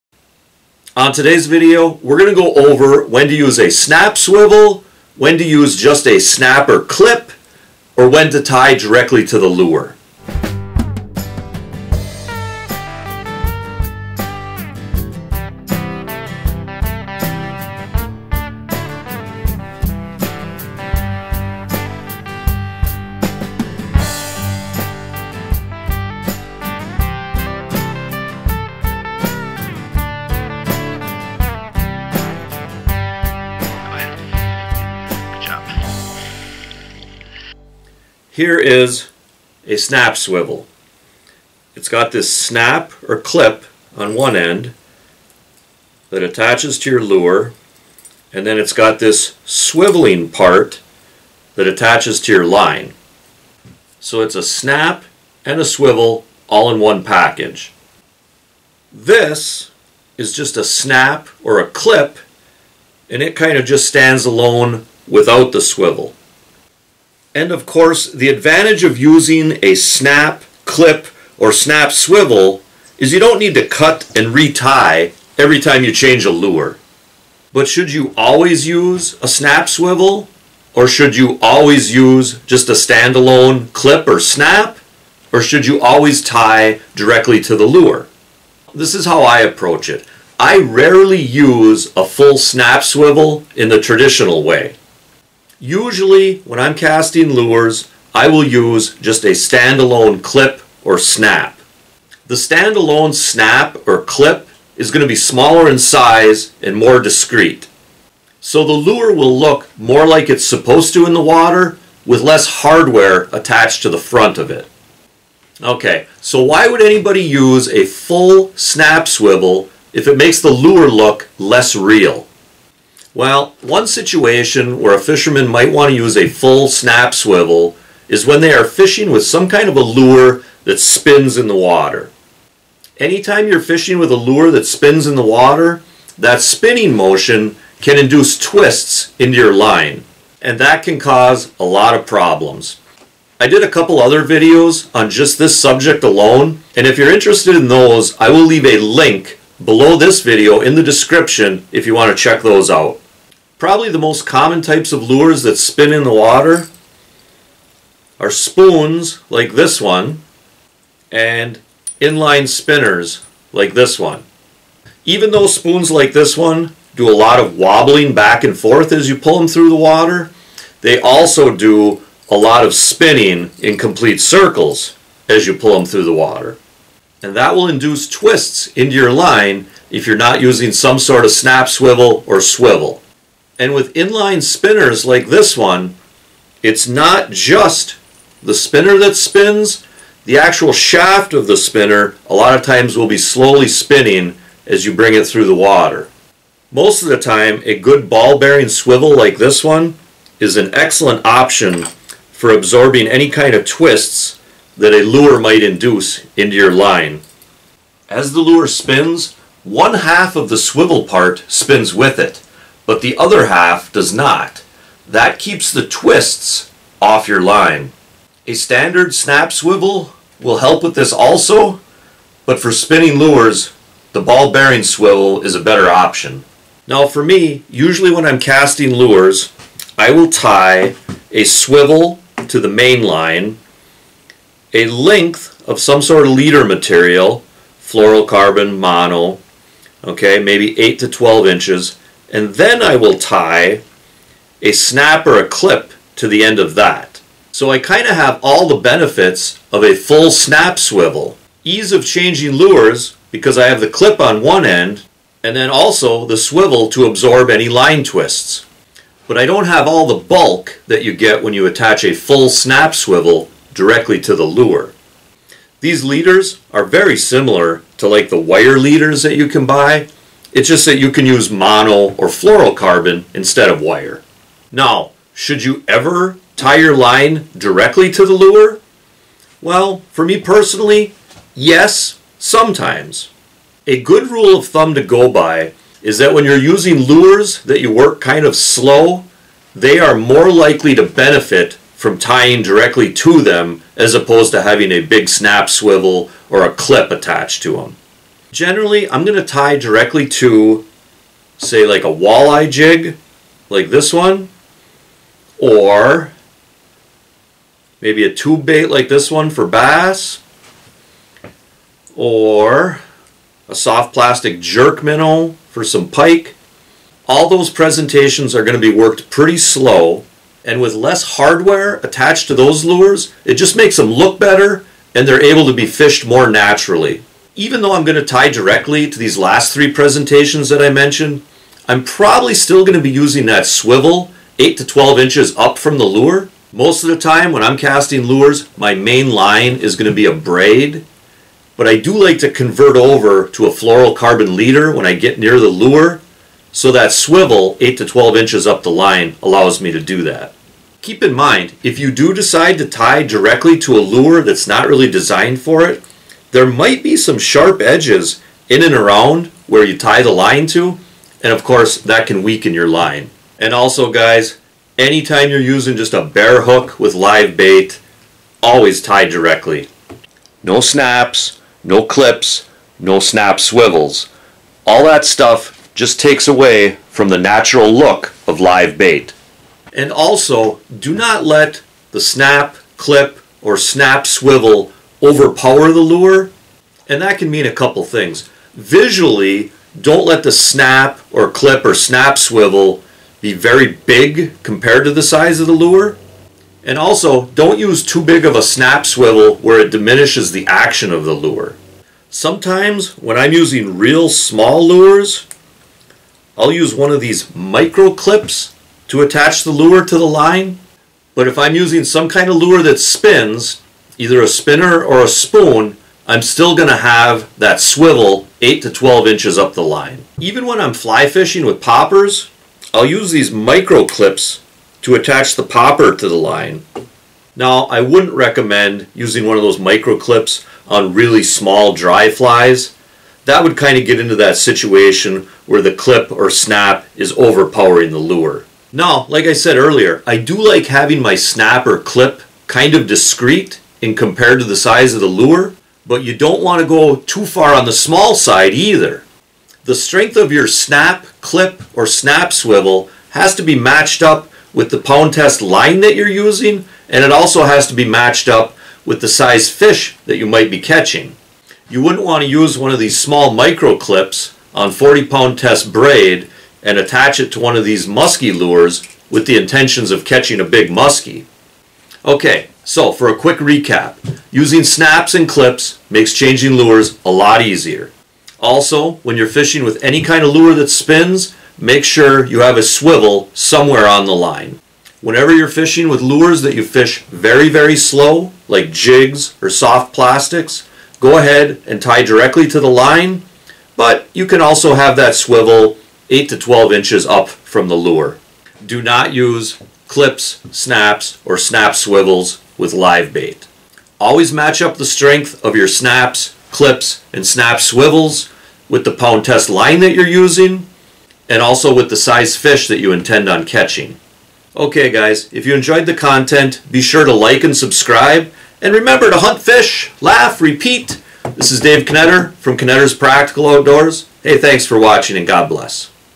How And When To Use A Snap Swivel, Clip Or Tie Directly
Catch more fish by using snap swivels and clips correctly.  Also, sometimes it's much better to tie directly to the lure.
SNAP SWIVELS
MICRO CLIPS